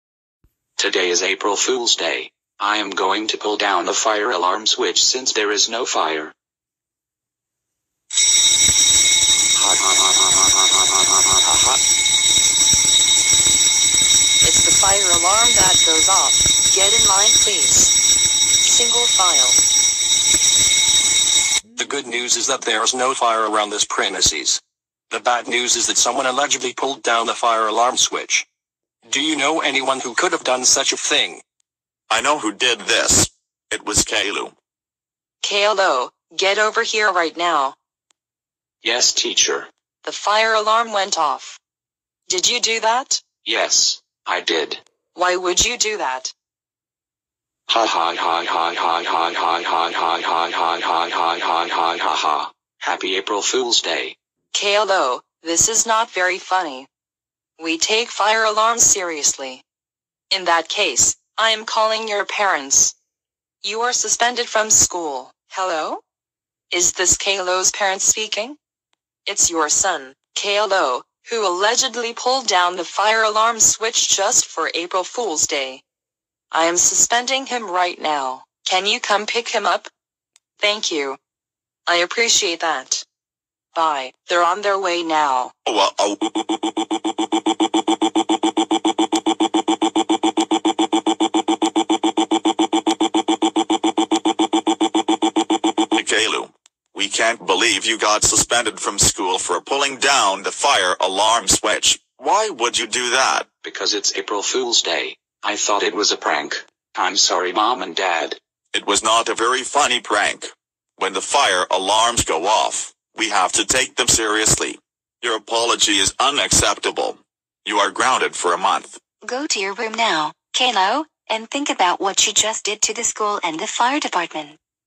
Caillou pulls the fire alarm
and gets grounded